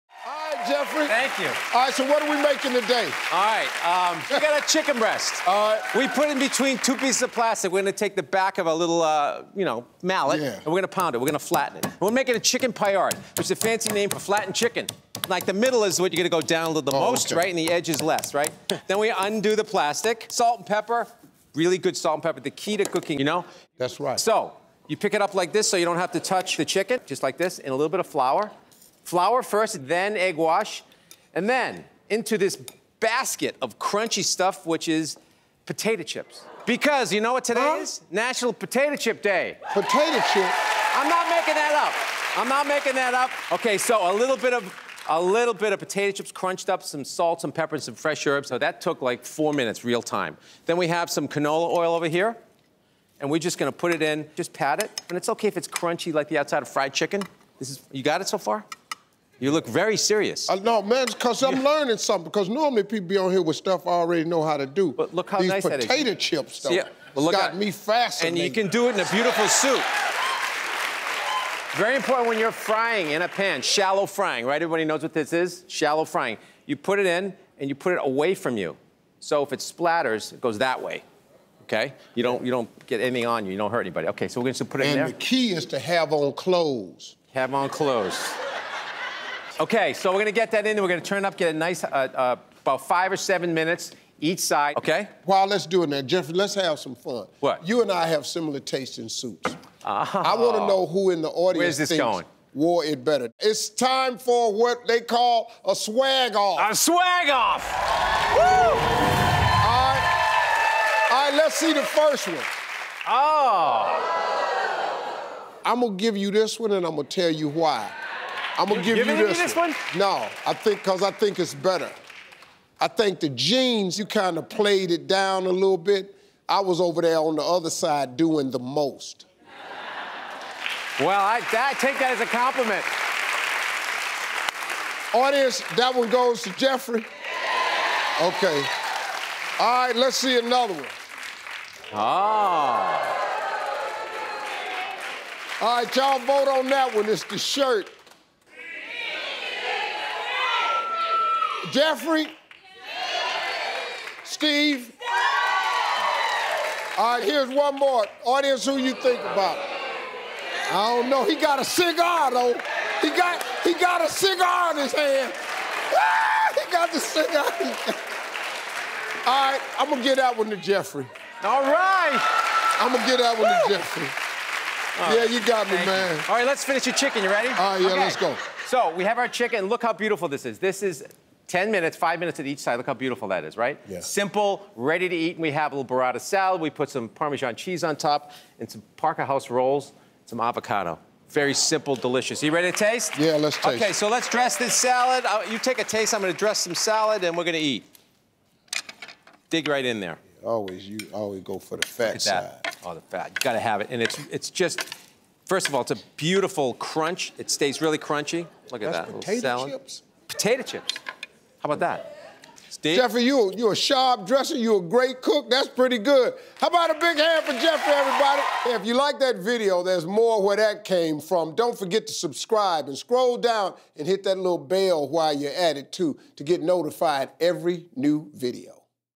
Chef Geoffrey Zakarian Puts WHAT On His Chicken?
Your kids will love this one- Geoffrey Zakarian's potato chip crusted chicken!  Plus, Steve Harvey has a little surprise for Geoffrey while they cook.